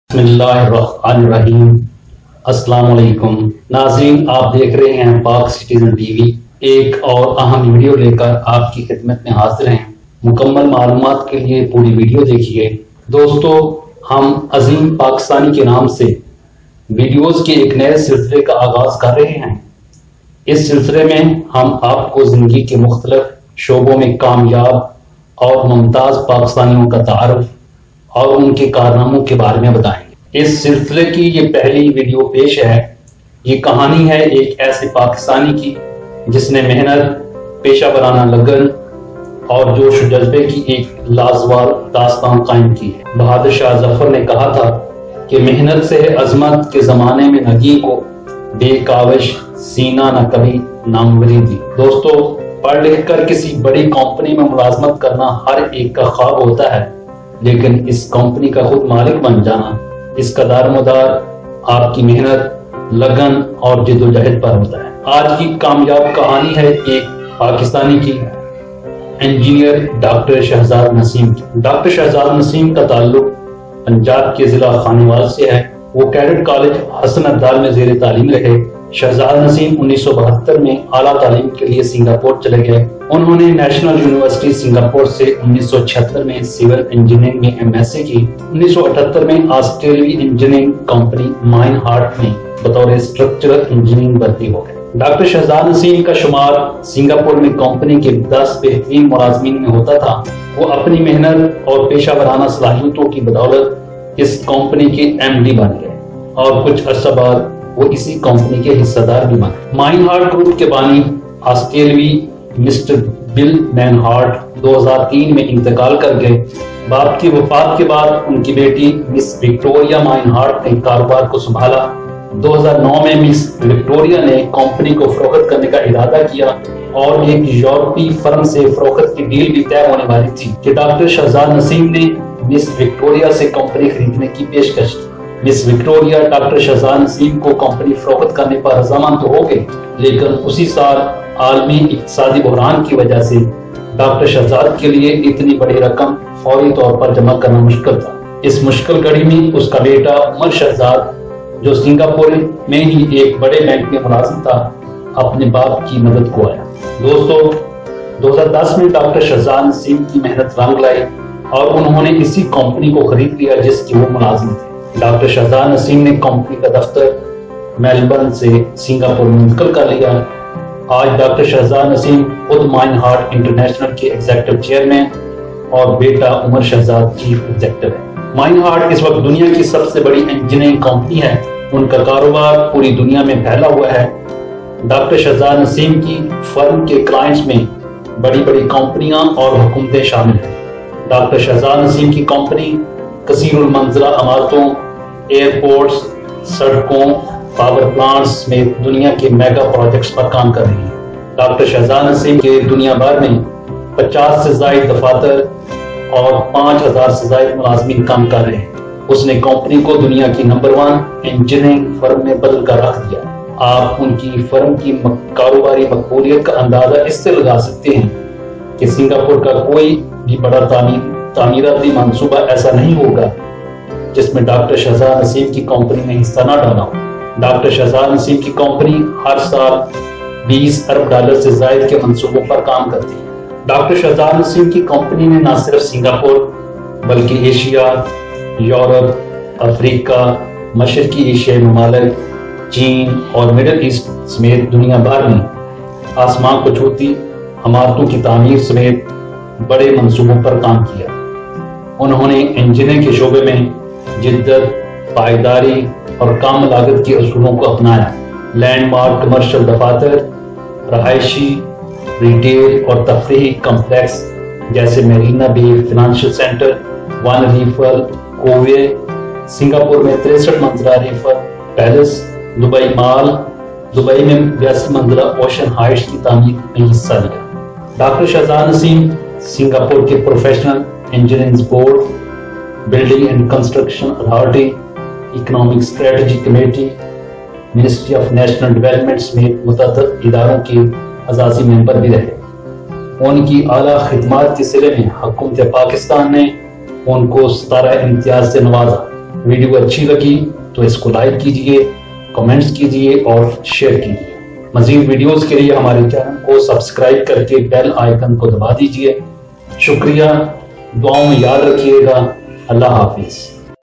Pakistani who bought the firm he works in | Success Story of a Pakistani Engineer | Pak Citizen TV
This video is about a successful Pakistani Structural Engineer who bought the world top Construction Firm. Dr Shahzad Nasim was employed as Structural Engineer in Meinhardt, an Australian Engineering Company in its Singapore office.
Dr. Shahzad Nasim belongs to Khanewal Punjab. Dr. Shahzad Nasim became its Managing Director and then its shareholder with his struggle, hardwork and honesty. He purchased the firm in 2010 after the death of its owner and founder Mr. Bill Meinhardt, an Australian based at Melbourne, Australia.
Dr. Shahzad Nasim is Executive Chairman and his son Mr. Omar Shahzad is CEO of the company. He made the firm of international repute. The company of Dr. Nasim has more than 50 offices the world over and has more than five thousand employees in his company. Dr. Shahzad Nasim is a wonderful success story.